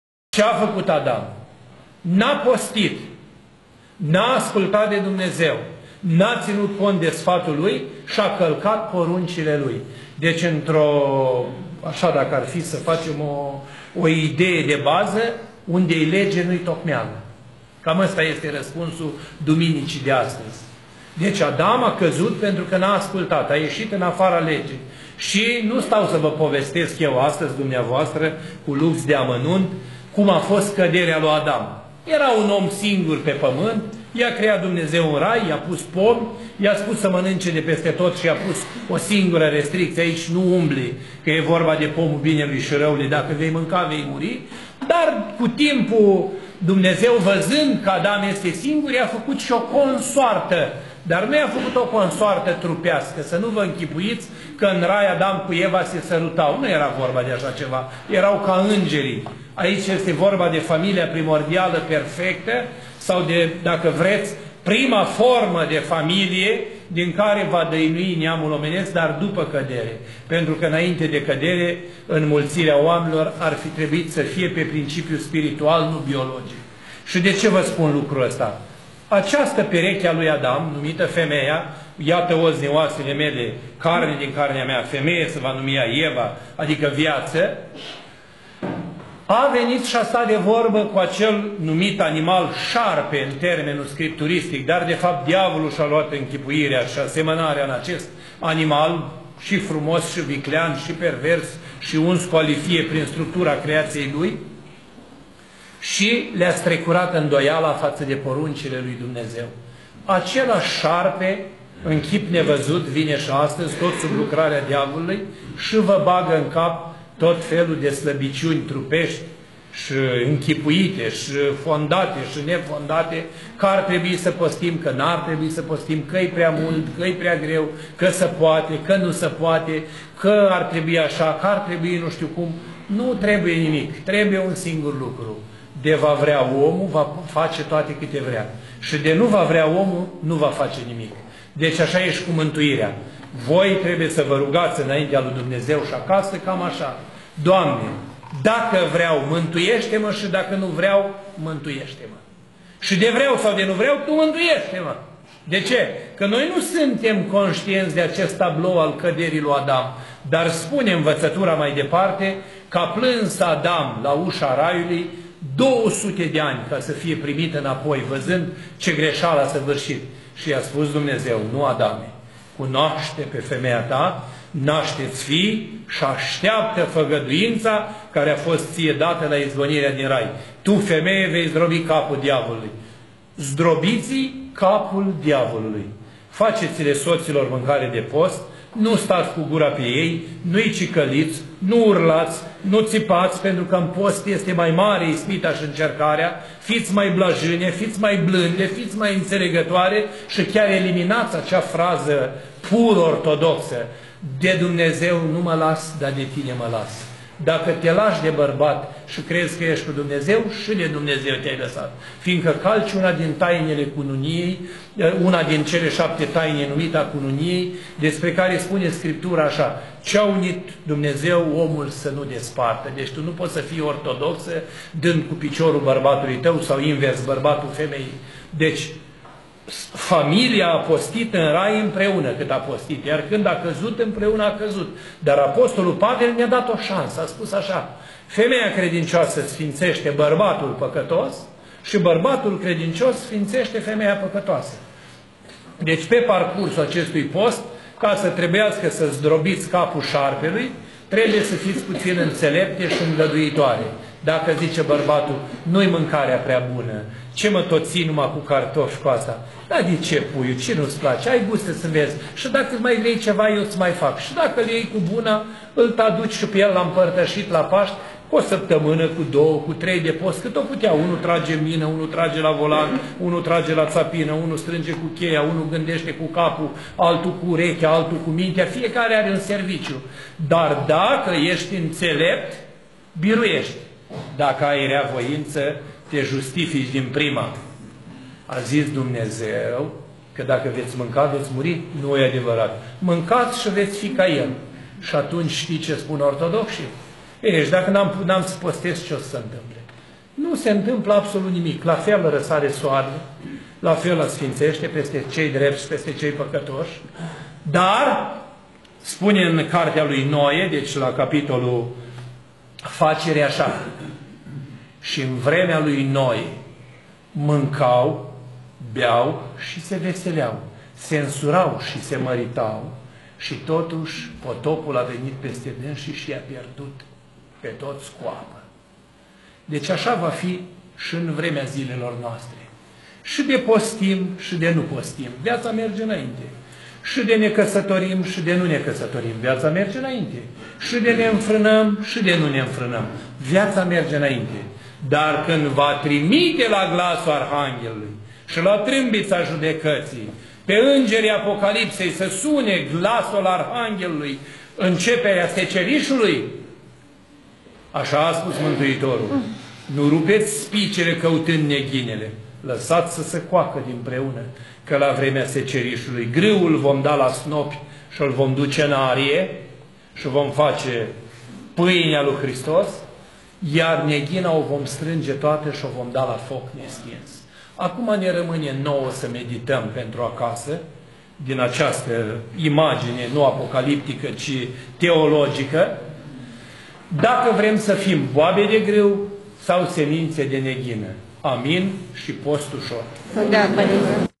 Pr. Calistrat: Eliminați acea frază pur ortodoxă: "De Dumnezeu nu mă las, dar de tine mă las!"
Faceţi soților mâncare de post, nu stați cu gura pe ei, nu-i cicăliți, nu urlați, nu țipați pentru că în post este mai mare ispita și încercarea. Fiţi mai blajine, fiţi mai blânde, fiţi mai înțelegătoare, și chiar eliminați acea frază pur ortodoxă: "De Dumnezeu nu mă las, dar de tine mă las!" Dacă te lași de bărbat și crezi că ești cu Dumnezeu, și de Dumnezeu te-ai lăsat. Fiindcă tu calci una dintre tainele cununiei, una dintre cele 7 despre care Scriptura spune așa: Ce a unit Dumnezeu, omul să nu despartă! Deci tu nu poți să fii ortodoxă dând cu piciorul la bărbatul tău sau invers. Familia a postit în rai împreună, și când a căzut, a căzut împreună! Apostolul Pavel ne-a dat o șansă: Femeia credinciosă, sfințește pe bărbatul păcatos și bărbatul credincios, sfințește femeia păcătoasă! Trebuie să fiți puțin înțelepte și îngăduitoare, atunci când bărbatul zice:
- Nu-i mâncarea prea bună, ce tot mă ții pe cartofi?
- Dar de ce puiu', ce nu-ți place? Hai gustă să vezi și dacă mai vrei ceva eu îți mai fac! Daca îl iei cu bunătate, îl aduci și pe el la împărtășit!